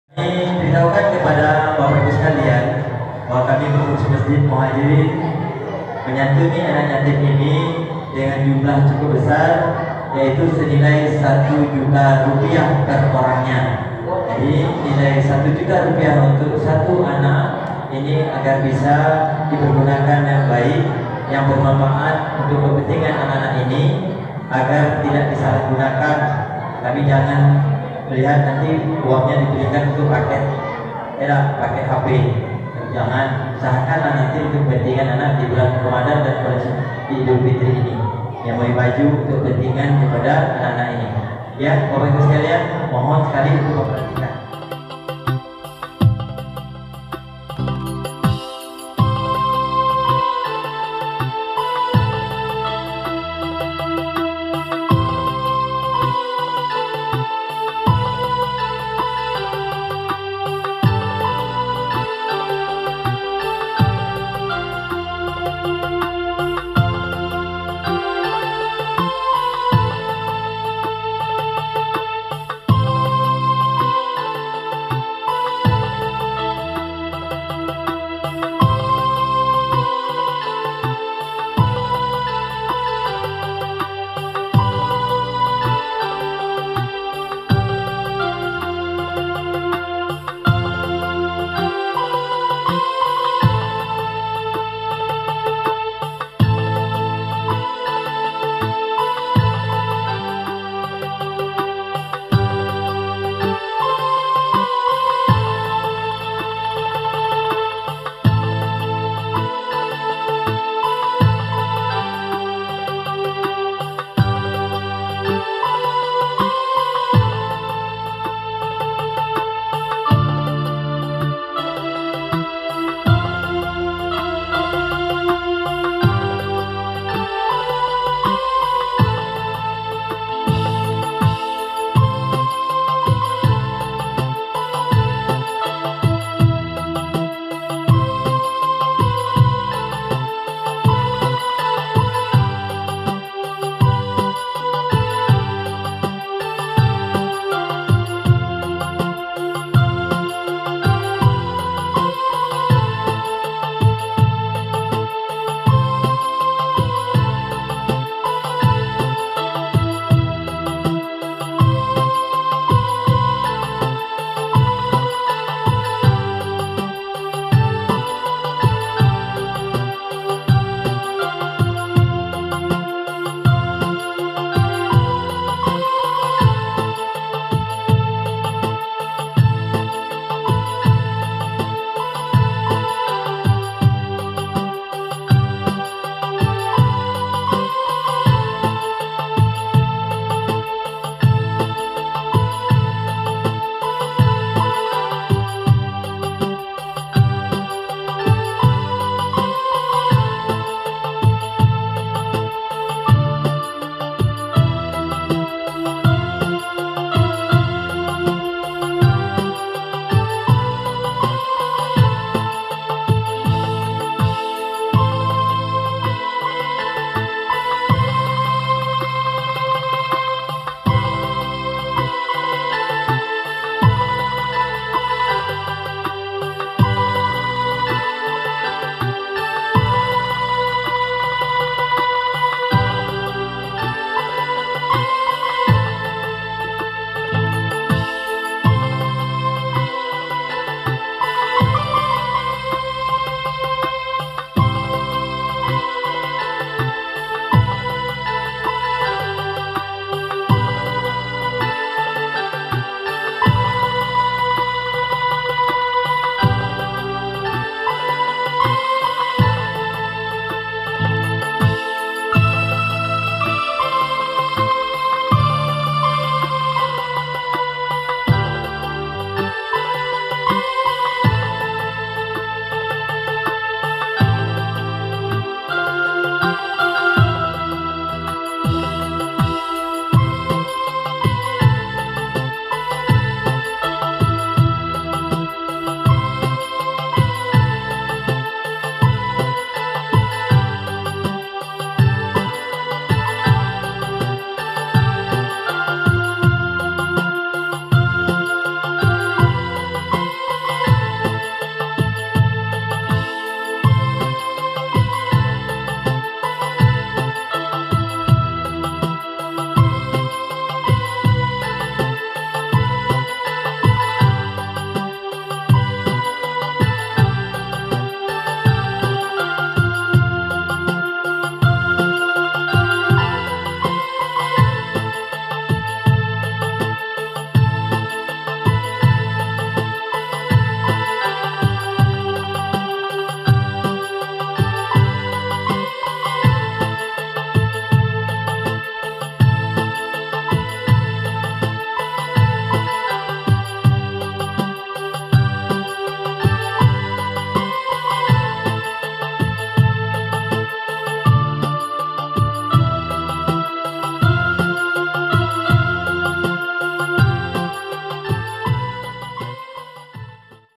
SANTUNAN ANAK YATIM 58 JUTA DIBAGIKAN OLEH PANITIA MASJID AL MUHAJIRIN PASAR PAMENANG 03 MEI 2021
SANTUNAN ANAK YATIM 58 JUTA
OLEH PANITIA MASJID AL MUHAJIRIN PASAR PAMENANG
SUMBER DANA DARI PARA JAMA'AH MASJID AL MUHAJIRIN PASAR PAMENANG
KEC. PAMENANG
KAB. MERANGIN
PROV. JAMBI
TERIMA KASIH KEPADA SEMUA DONATUR DAN PANITIA
SEMOGA ALLOH MEMBALAS SEMUA AMAL IBADAH SAUDARA SAUDARI SEKALIAN AMIN